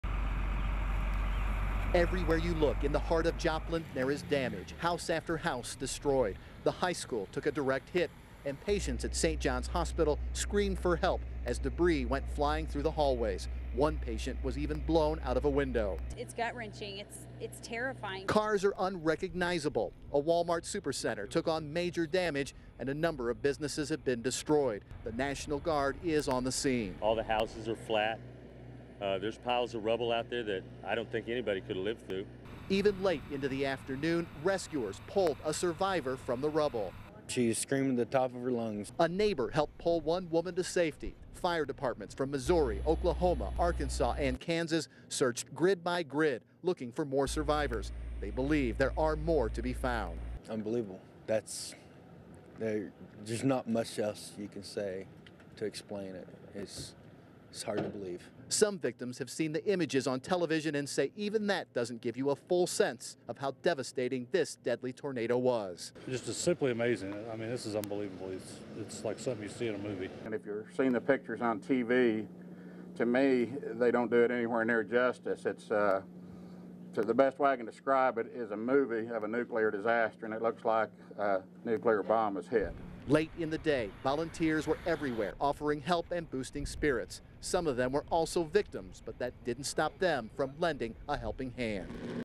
Joplin EF5 day 1
This was one of the toughest assignments I have had in my career. My hometown of Joplin Missouri was leveled by a massive tornado on May 22, 2011. This is an example of the devastation I witnessed when the sun broke the morning after.
Shot and edited for KTVI FOX 2 St. Louis and Local TV LLC.